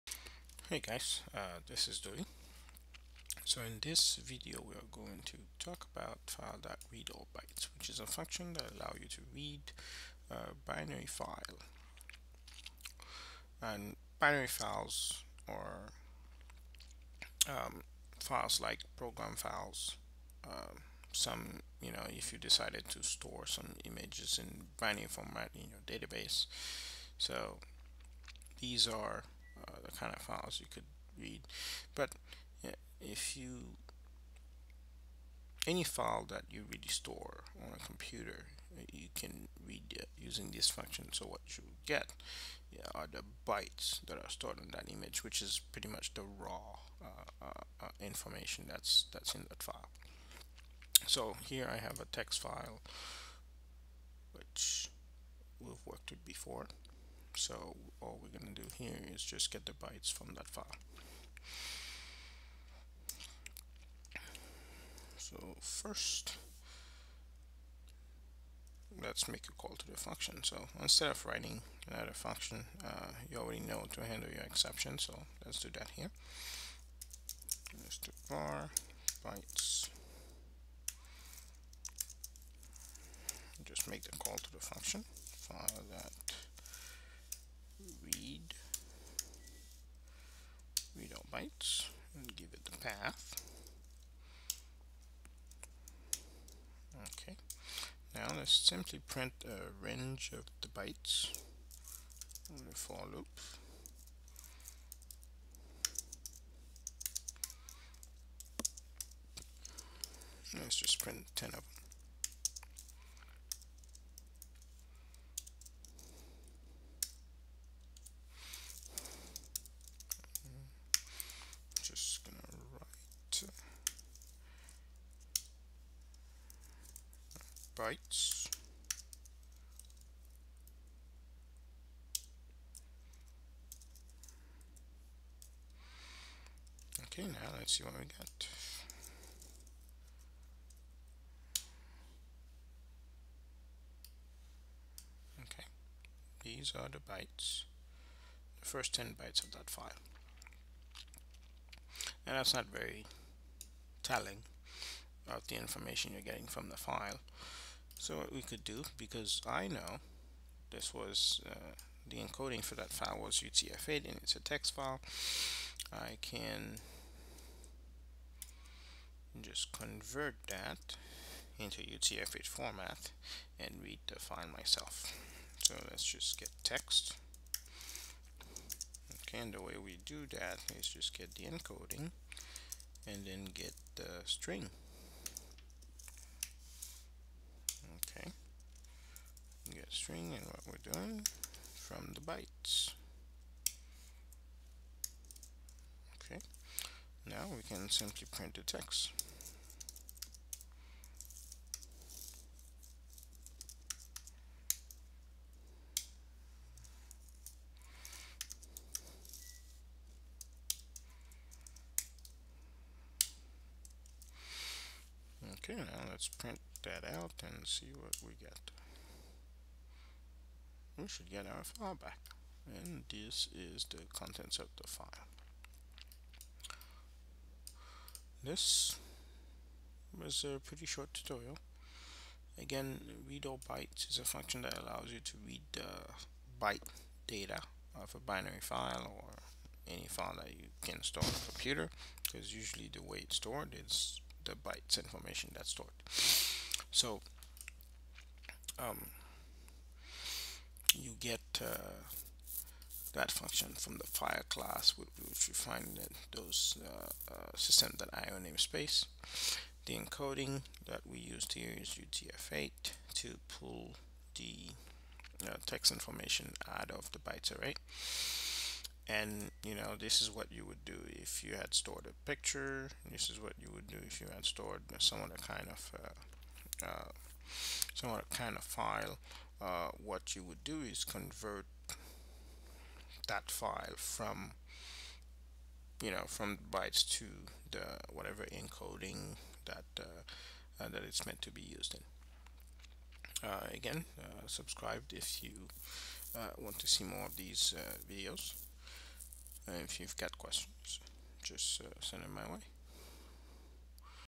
ReadAllBytes Tutorial
Read binary data from file; in this specific case, read the bytes from a text file and get the text content of the file using the known encoding of the file (UTF8) and the GetString() function